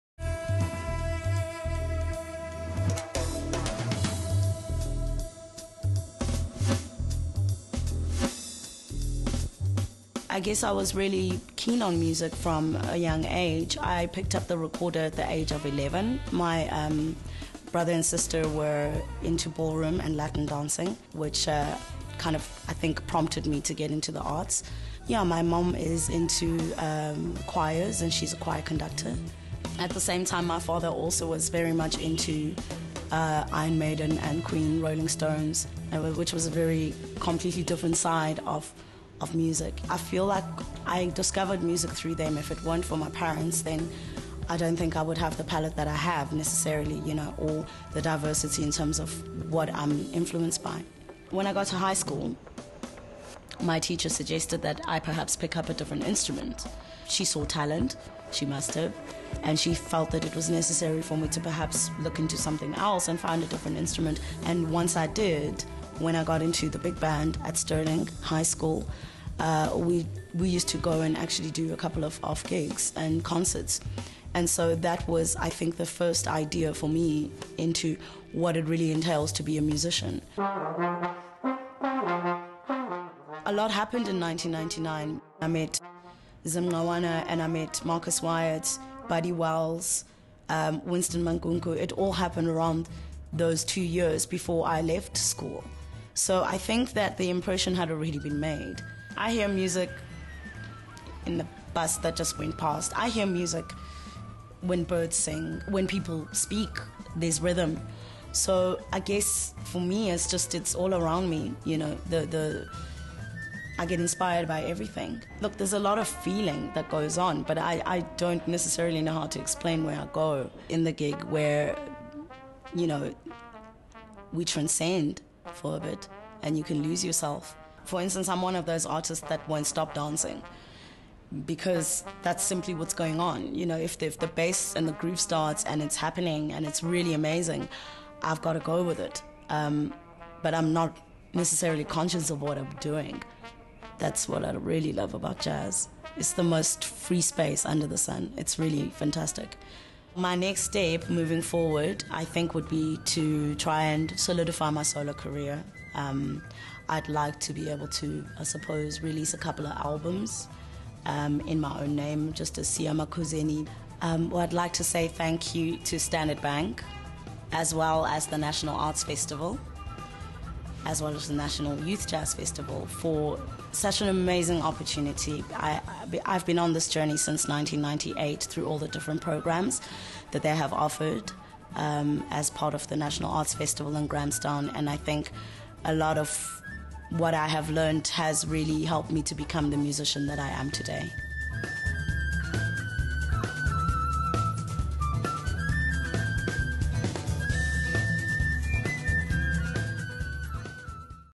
Standard Bank Young Artist Award Winners for Jazz SIYA MAKUZENI
Siyavuya Makuzeni is a trombone player, vocalist, lyricist and songwriter well known for her uniquely experimental, edgy, yet pure intonation. Born in 1982 in the Eastern Cape, Siya grew up singing in the choir and playing recorder before picking up the trombone while at Sterling High School in East London.

‘The influence of Xhosa music and jazz is the foundation of my musical beginnings, and has helped me to shape my own voice or individual expression,’ she told Struan Douglas in The Story of South African Jazz.